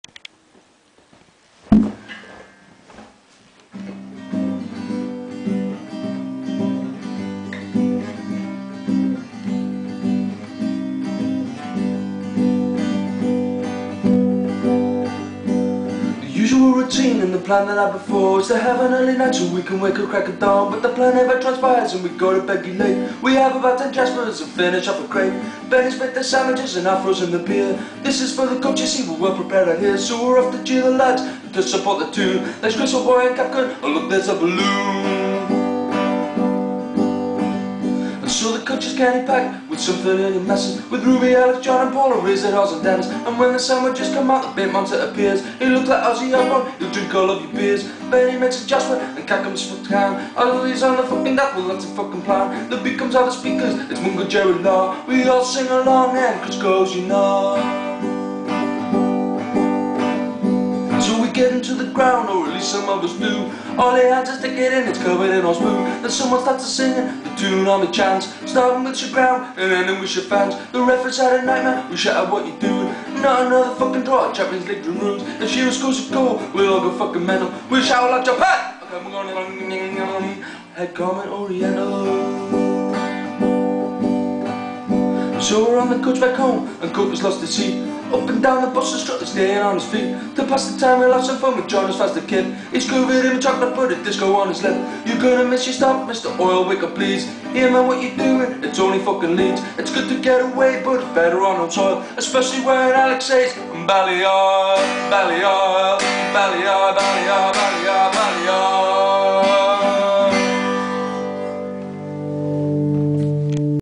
Ballyoil
The usual routine and the plan the night before
is to have an early night so we can wake up crack of dawn
but the plan never transpires and we go to bed geet late
we have about 10 jaspers and finish off a crate.

Benny's made the sandwiches and I've frozen the beer
this is for the coach you see we're well prepared right here
so we're off to cheer the lads, and to support the toon
there's Chris Oilboy and Capgun, oh look there's the baloon.

So the coach is canny packed, with some familiar messes
With Ruby Alex John and Paul, or is it Oz and Dennis?
and when the sandwiches come out, the bait monster appears
he looks like Ozzy Osbourne, he'll drink all of your beers.

Benny makes a jasper, and Capgun sparks a can
"are yuz on the fuckin death?" - "well that's the fucking plan"
the beat comes out the speakers, it's Mungo Jerry la
we all sing along, and Chris goes "yu kna".

So we get into the ground, or at least some of us do
Oily hands his ticket in, it's covered in horse poo
then someone starts the singing, and the toon army chants
starting with "shit ground", and ending with "shit fans".

The ref has had a nightmare, we shout out "what you doing?"
not another fucking draw, our champions league dreams ruined
if Shearer scores a goal, we'll all go fucking mental
we'll shout out loud "JAPHAT", head garment, oriental.

So we're on the coach back home, and Corker's lost his seat
up and down the bus he'll strut, he's staying on his feet
to pass the time he'll have some fun, when John is fast a kip
he's covered him in chocolate, and put a disco on his lip.

"You're gonna miss your stop, Mr Oil wake up please!"
"here man what you doing? it's only fucking Leeds!"
it's good to get away, but it's better on home soil
especially when Alex says. BALLOIL!!!

copyright - Ballyoil